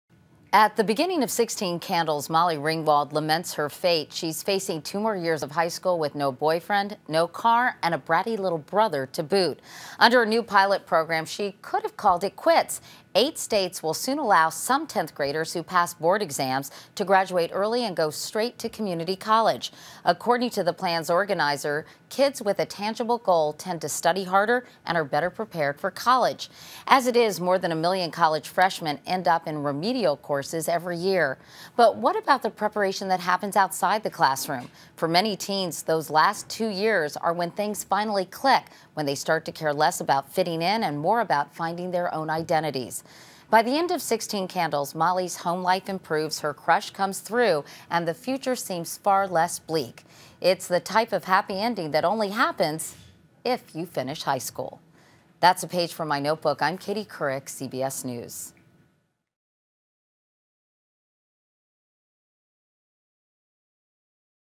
Notebook: Early College
Eight states will soon allow some tenth graders who pass board exams to graduate early. Katie Couric comments that, for many teens, those last two years can be when things finally click.